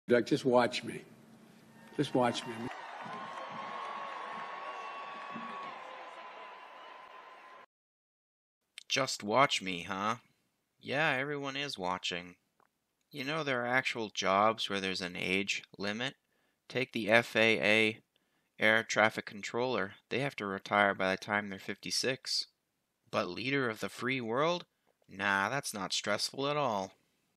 Watching the Fall🙄🤔
Joe Biden 'fine' after fall on stage in Colorado, yeah ok. Well here's my thoughts on it. Link used.